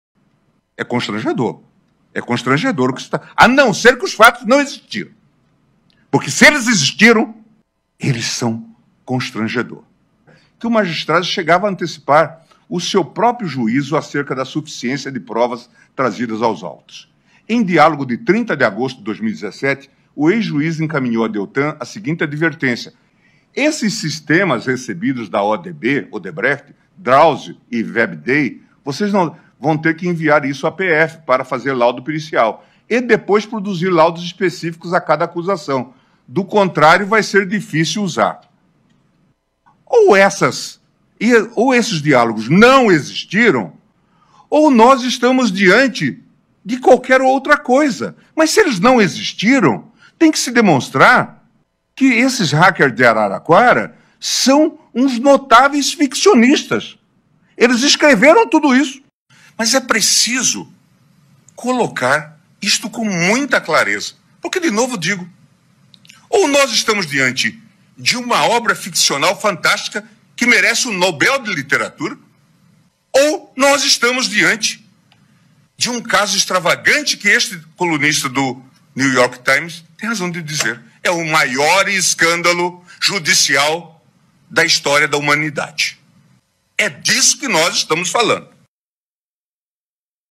Veja o trecho censurado pelo Jornal Nacional da fala de Gilmar Mendes
CENSURADO NO JORNAL NACIONAL: VEJA O TRECHO DA FALA DE GILMAR QUE HUMILHOU MORO E DALLAGNOL!!!